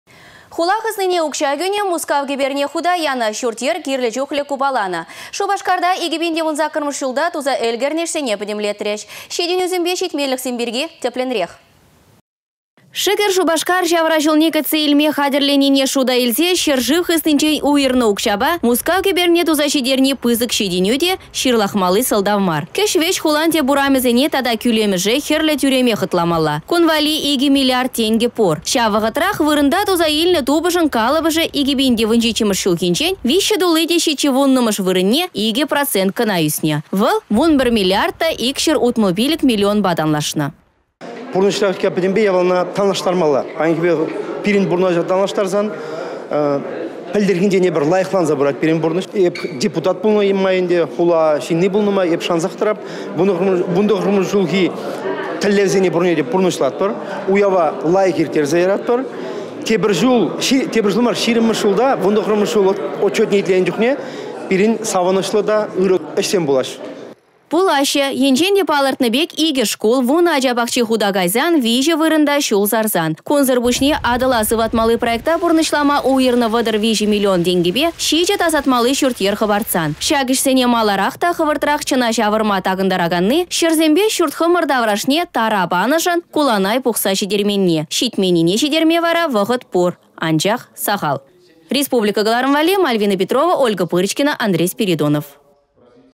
«Çитĕнӳсемпе çитменлĕхсем калаçтараççĕ»
Хула хыснине укçа кӳнĕ, Мускав кĕперне хута янă, çурт-йĕр кирлĕ чухлĕ купаланă. Шупашкарта 2018-мĕш çулта туса ĕлкĕрнĕ ĕçсене пĕтĕмлетрĕç. Çитĕнӳсемпе çитменлĕхсем пирки - тĕплĕнрех.